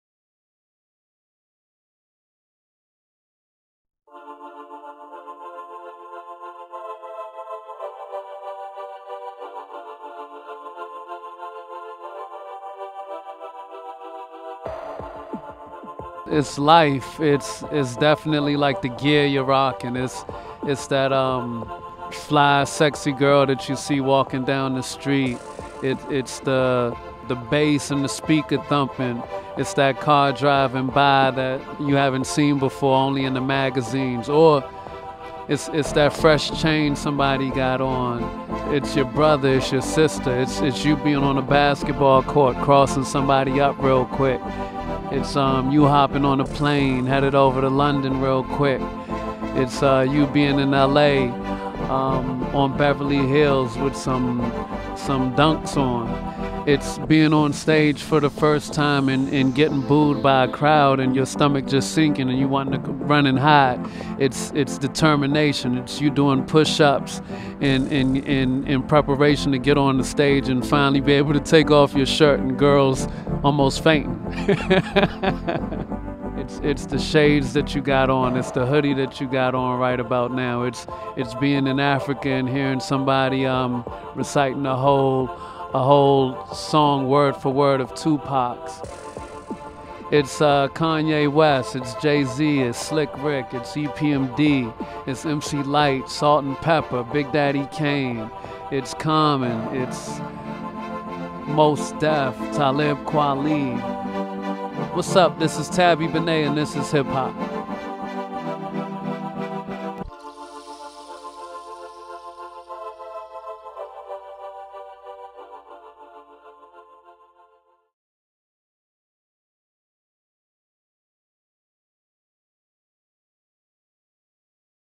Tabi Bonney: This Is Hip Hop [Directed by Court Dunn]
ThisIs50.com and Director Court Dunn present the ninth installment of the THISISHIPHOP video series featuring TABI BONNEY. Theme music produced by Reality.